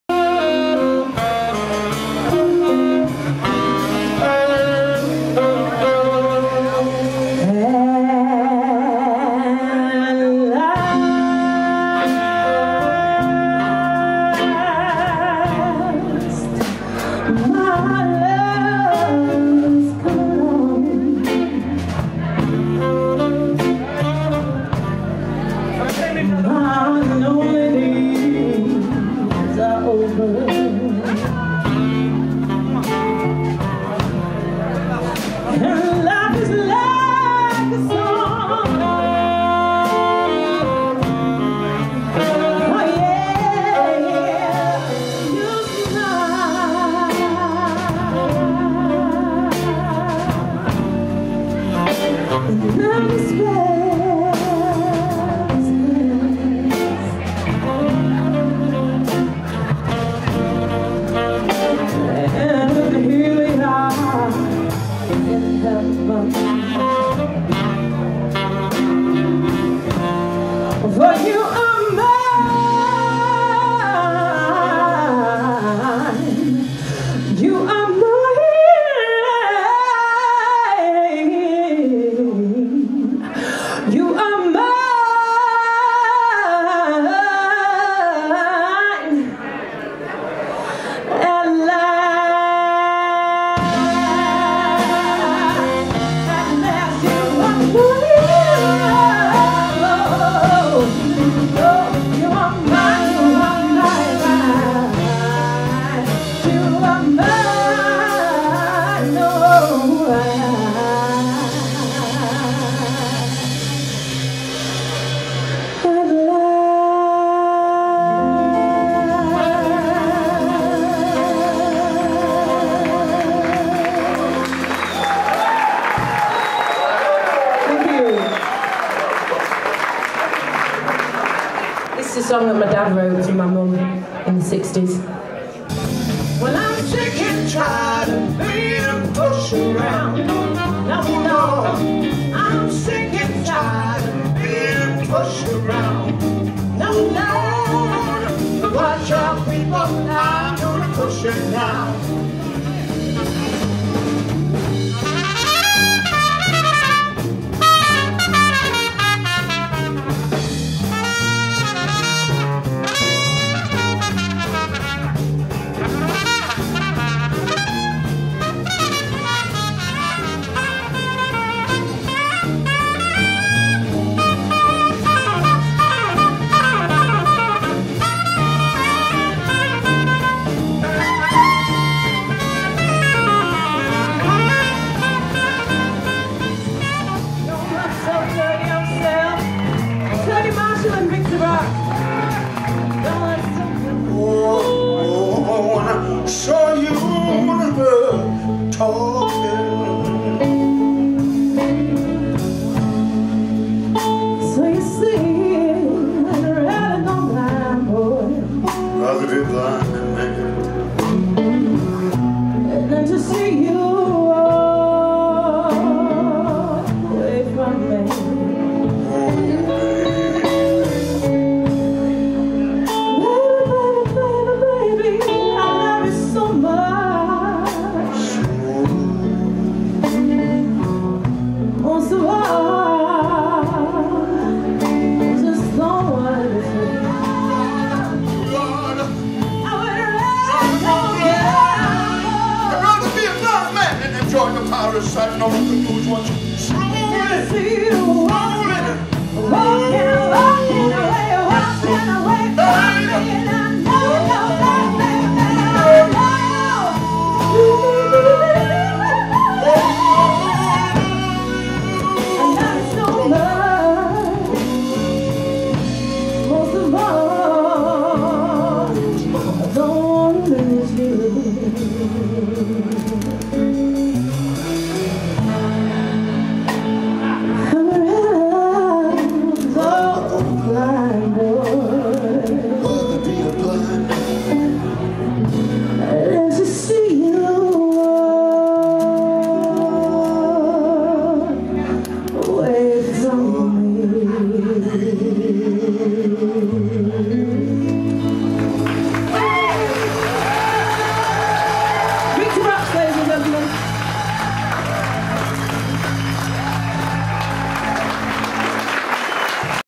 Stockport Beer festival 2014. 'The Kyla Brox Band'
Saturday night at Stockport Beer Festival. The Kyla Brox Band.